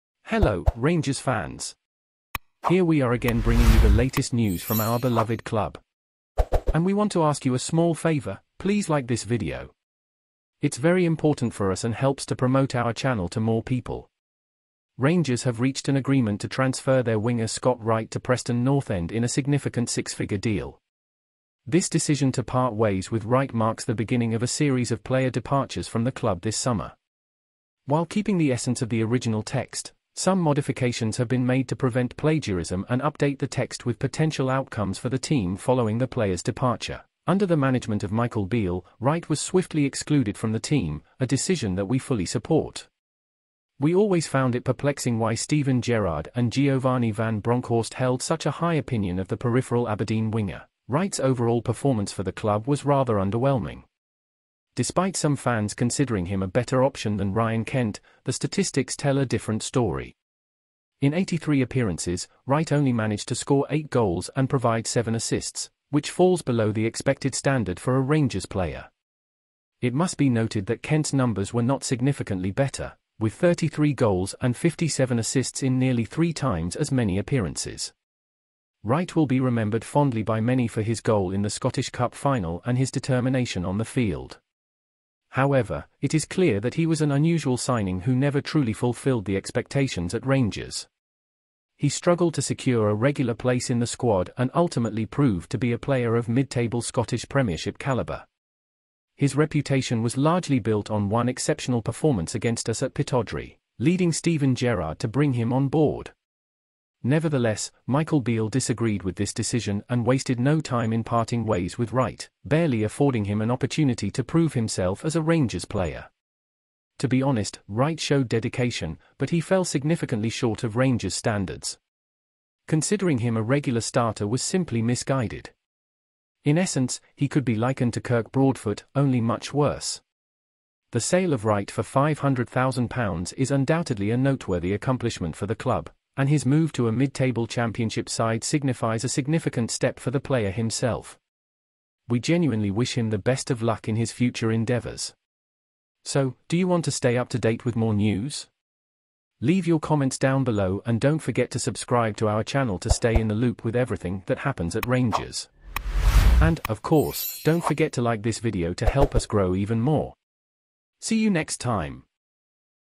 🔥 RANGERS' SHAKE-UP BEGINS! 💰SIX-FIGURE TRANSFER! 🔵⚪️RANGERS FC NEWS TODAY
🔵⚪️✍️ Rangers have sealed a major deal! Winger Scott Wright is heading to Preston North-End in a significant six-figure transfer. This move marks the beginning of a summer shake-up at the club.

🗣️ Under Michael Beale's management, Wright swiftly fell out of favor, a decision we fully support. It always puzzled us why Gerrard and van Bronckhorst rated him so highly at Aberdeen. 🤔

⚽️ Overall, Wright's performance at Rangers was underwhelming. Despite some fans favoring him over Ryan Kent, the numbers paint a different picture. In 83 appearances, he scored only 8 goals and provided 7 assists, falling short of Rangers' expectations.

👋 Wright will be remembered for his Scottish Cup final goal and determination on the pitch. However, it's evident he never quite lived up to the Rangers standard. His reputation was built on one standout performance, leading to his signing, but Beale disagreed and quickly moved on.

⚡️ To be frank, Wright's dedication couldn't compensate for his lackluster performance. Starting him regularly was a misjudgment, as his level was far below what Rangers needed. He could be likened to Kirk Broadfoot, but even worse.